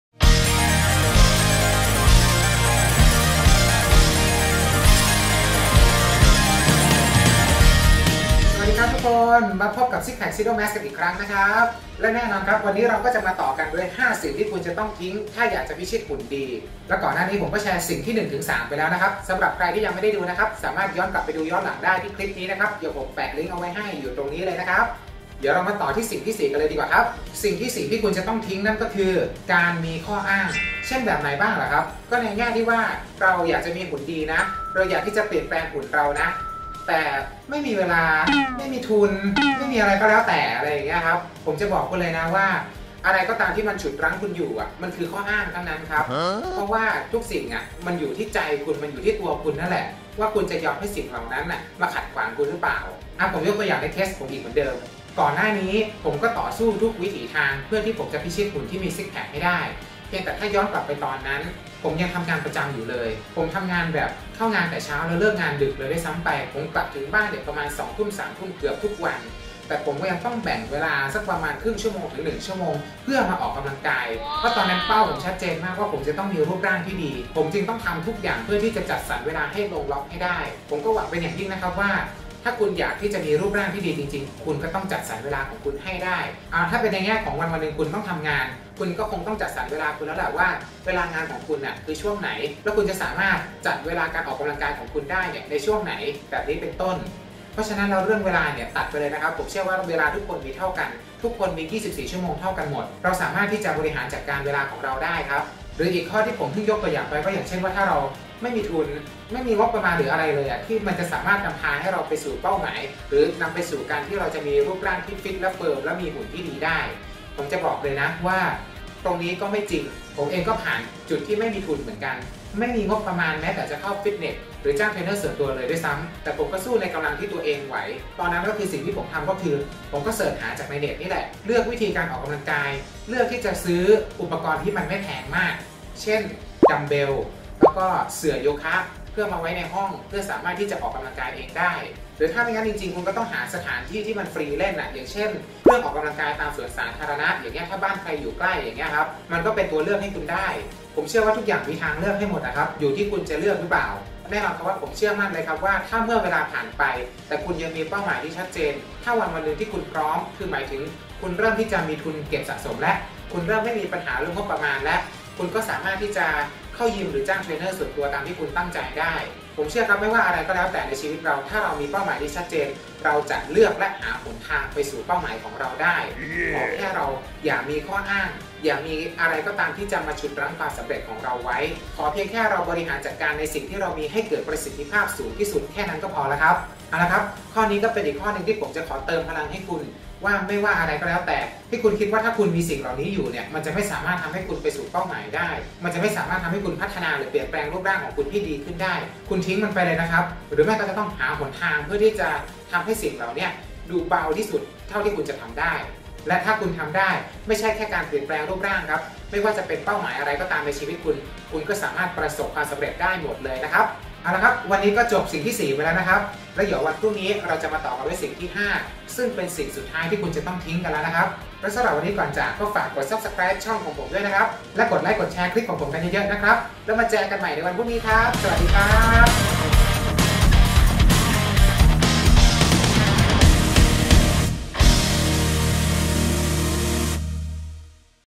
5 สิ่งที่คุณต้องทิ้ง ถ้าอยากจะมีหุ่นดี (ตอนที่ 4) | SixPackXedo Mask
แนวคิดสุดเจ๋งให้เป็นของขวัญปีใหม่อีกหนึ่งชิ้น ให้ทุกคนได้รู้ก่อน เพื่อที่จะได้นำไปปรับใช้และพัฒนารูปร่างตัวเองได้ ผมรวบรวมเป็น 5 สิ่งที่คุณต้องทิ้งไปเสีย ถ้าอยากจะมีรูปร่างที่ Perfect มีอะไรบ้างนั้นมาฟังกันเลยครับ